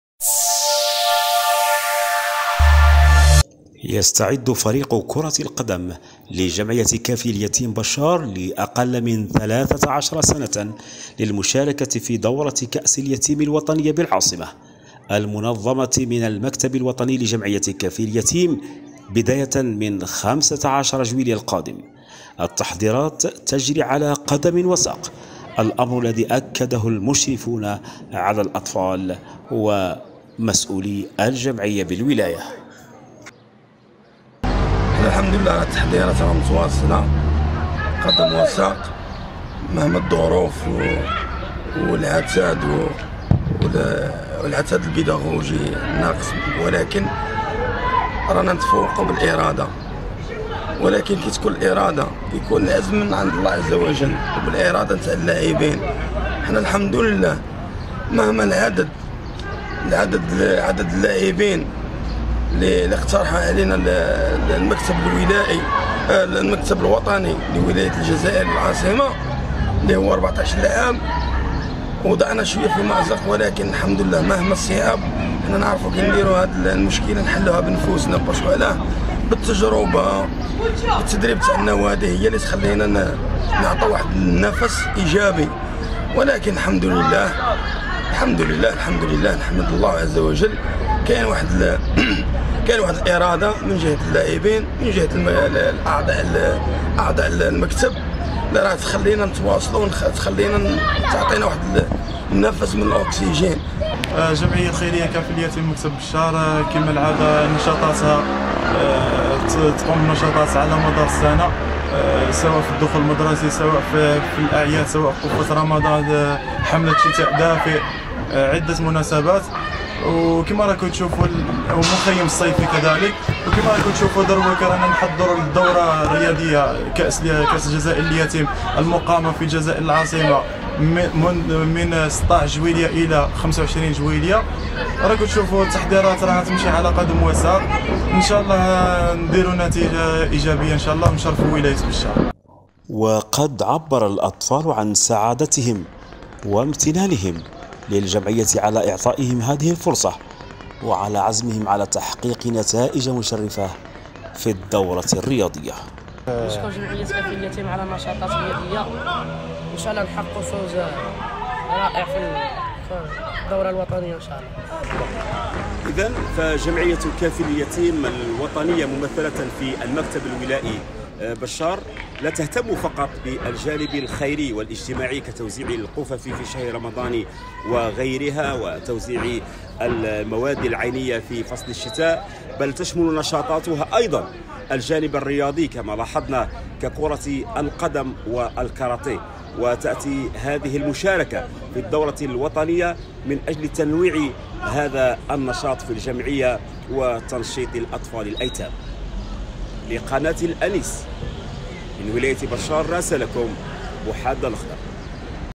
بشار: فريق كرة القدم أصاغر لجمعية كافل اليتيم يحضر للمشاركة في كأس الجزائر لليتيم
5000
فيسبوك :